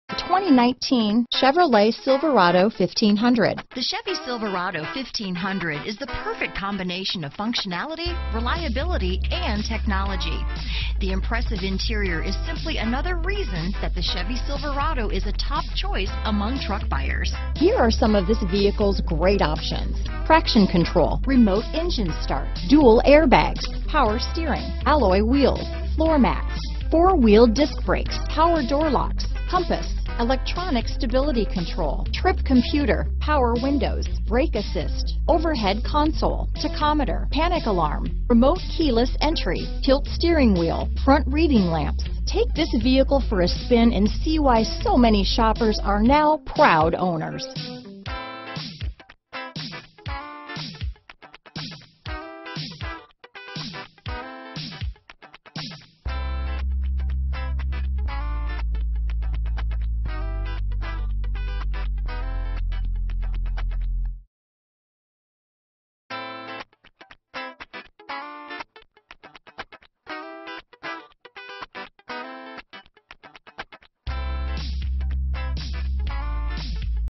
2019 Chevrolet Silverado 1500 Ocala, Gainesville, Leesburg, The Villages, Crystal River, FL TK40782
Black N 2019 Chevrolet Silverado 1500 available in Ocala, Florida at Palm Chevy Ocala. Servicing the Gainesville, Leesburg, The Villages, Crystal River, FL area.

4WDbrbrRecent Arrival!